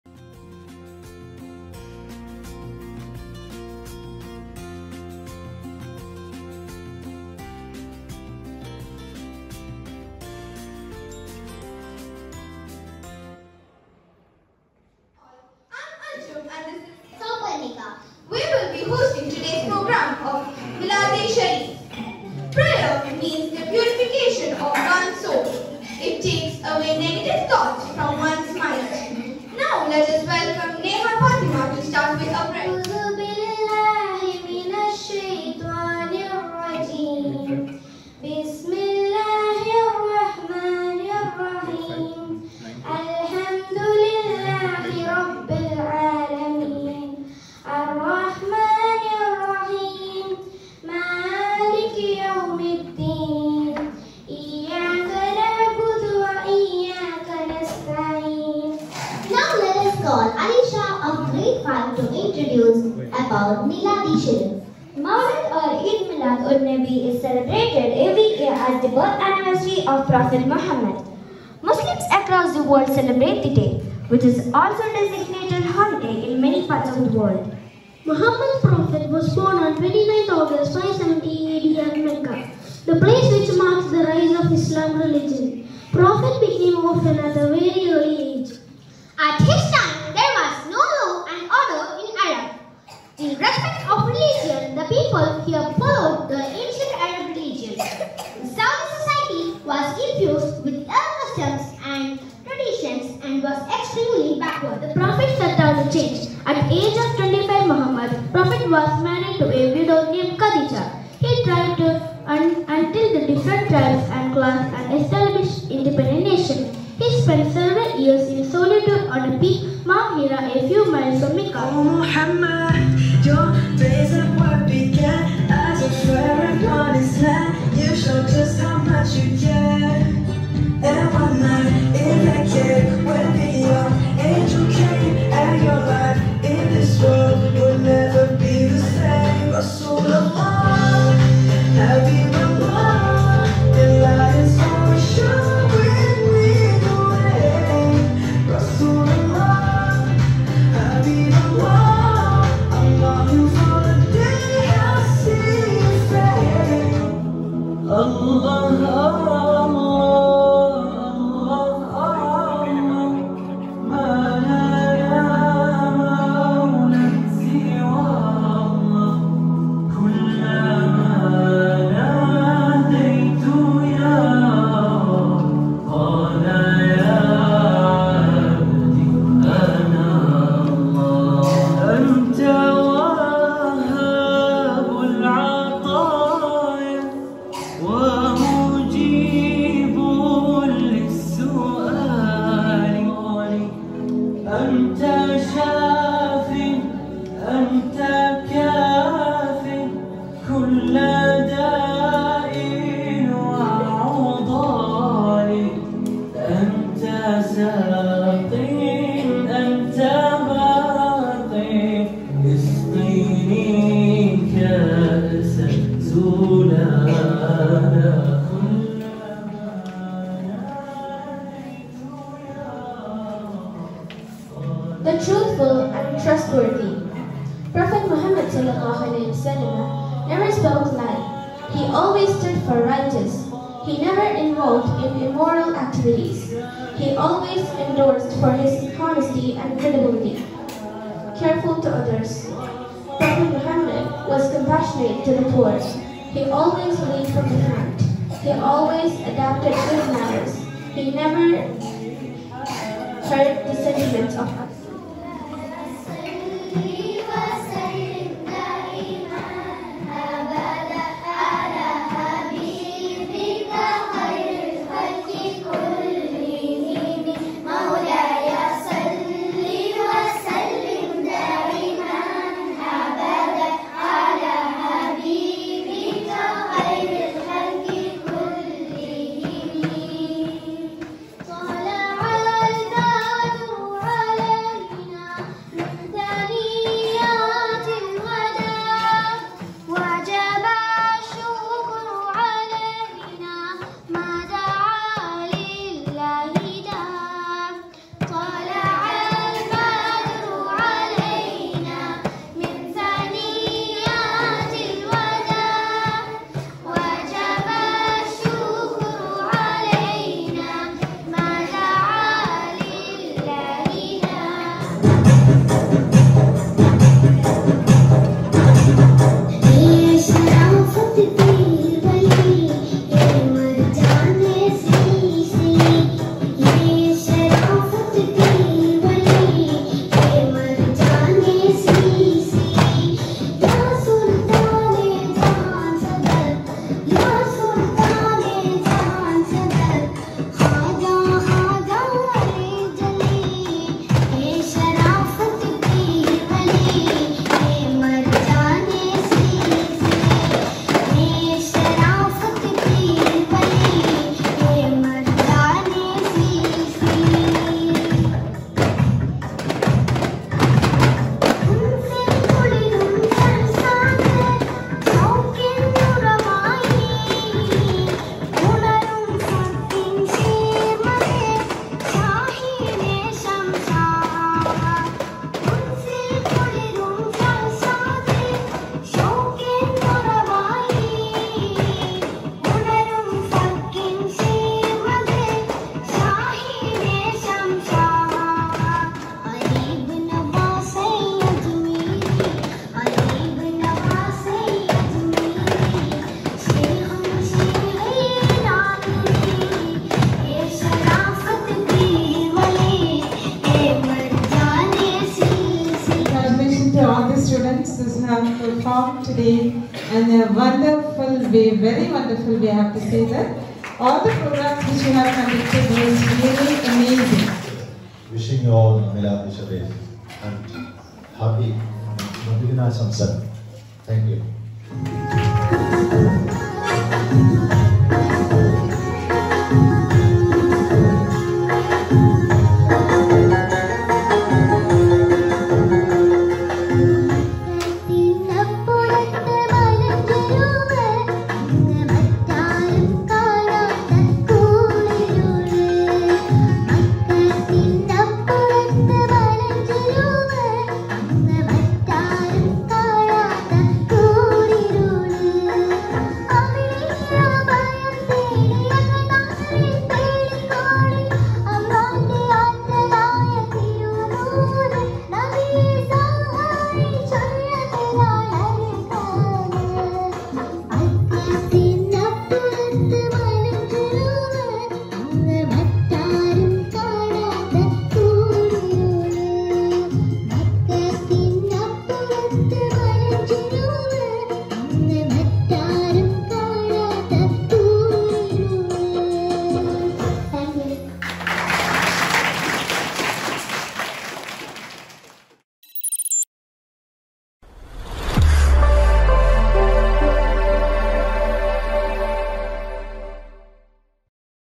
Milad e Shareef Celebrations 2022 - The Guardian English School
Here’s a sneak into The Guardian English School’s Milad e Sherif celebration. The 1497th birthday of Prophet Mohammed is a moment of great religious fervour. He is the last of the Islamic prophets, and people worldwide remember him for his strength and patience and seek his blessings. This pious moment at school was marked by the presence of our dear parents and teachers.